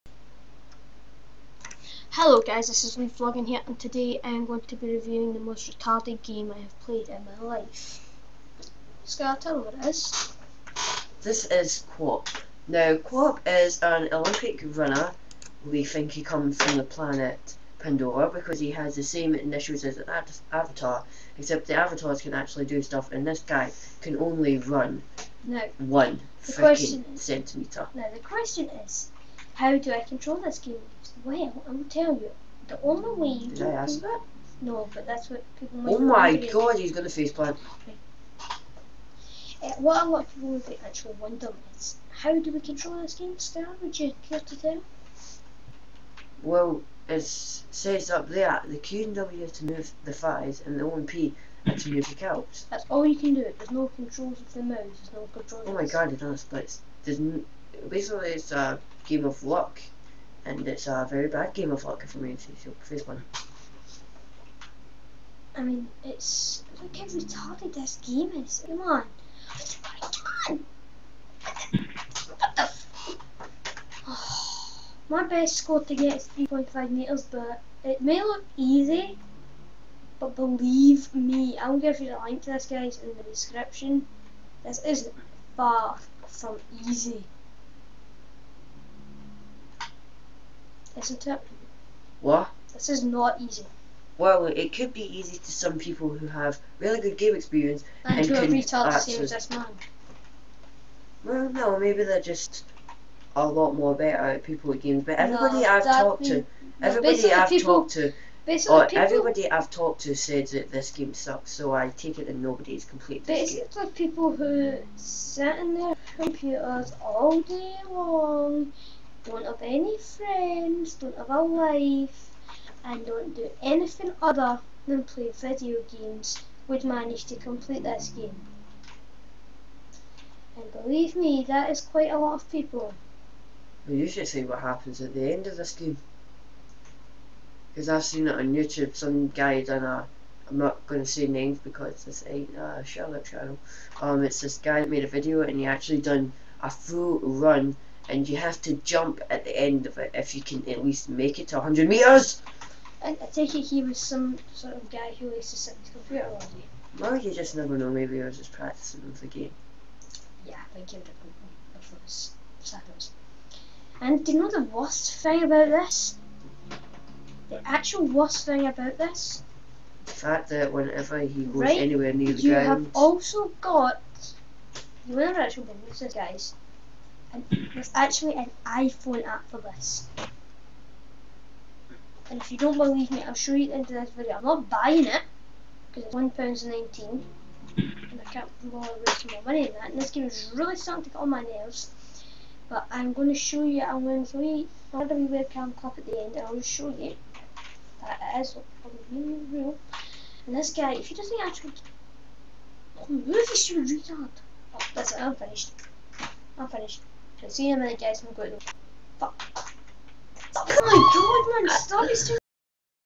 LeebzVlogging's Review of QWOP : "The Worst game ever".
Just go onto the AppStore and type in QWOP it is there. I don't recomened you buy it though as it sucks so badly!